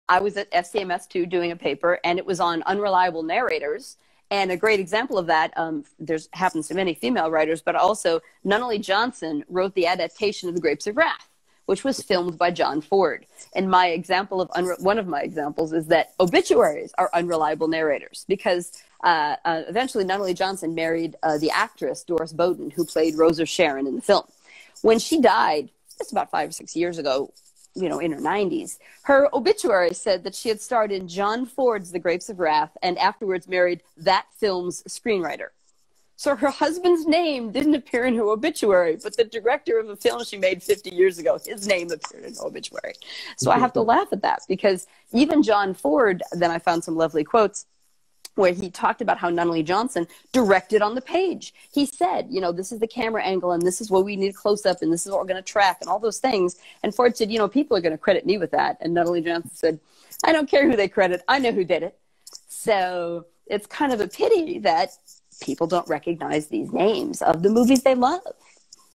03 Unreliable Narrators from In Conversation with Dr. Rosanne Welch: Screenwriter
Transcript:

I was at SCMS too, doing a paper and it was on Unreliable Narrators and a great example of that -- this happens to many female writers -- but also Nunnally Johnson wrote the adaptation of The Grapes Of Wrath which was filmed by John Ford and my example of -- one of my examples is that obituaries are unreliable narrators because eventuallyNunnally Johnson married the actress Doris Bowden who played Rosa Sharon in the film. When she died just about five or six years ago you know in her 90s,  her obituary said that she had starred in John Ford's The Grapes Of Wrath and afterward married that film's screenwriter. So her husband's name didn't appear in her obituary but the director of the film she made 50 years ago -- his name appeared in her obituary. So i have to laugh at that because even John Ford -- then I found some lovely quotes where he talked about how Nunnally Johnson directed on the page. He said you know this is the camera angle and this is what we need close up and this is all going to track and all those things and Ford said you know people are going to credit me with that and Nunnally Johnson said I don't care who they credit. I know who did it. So it's kind of a pity that people don't recognize these names of the movies they love.

One of the benefits of attending conferences is that you can meet the editors from the companies that have published some of your books face to face.  That happened at the recent SCMS conference where I met Intellect editor James Campbell and he invited me to be a guest on his InstagramLive show.  We chatted about my work with the Stephens College MFA in TV and Screenwriting, and then my work with co-editor Rose Ferrell on the Journal of Screenwriting’s special issue on Women in Screenwriting (Volume 11, Number 3) that came out recently and which featured articles about an international set of female screenwriters from Syria, Argentina, China and Canada (to name a few).  We even had time to nerd out on our own favorite classic films across the eras which brought up fun memories of Angels with Dirty Faces, Back to the Future, Bonnie and Clyde, and of course, all things Star Wars from the original 3 to The Mandalorian. It’s always so fun to talk to fellow cinephiles.

With Intellect Books Editor James Campbell (@IntellectBooks)

Speaking with Dr. Rosanne Welch @DrRosanneWelch) Author, teacher, and television screenwriter. Today we cover everything from women in screenwriting to our favorite Jimmy Cagney movies and Friends.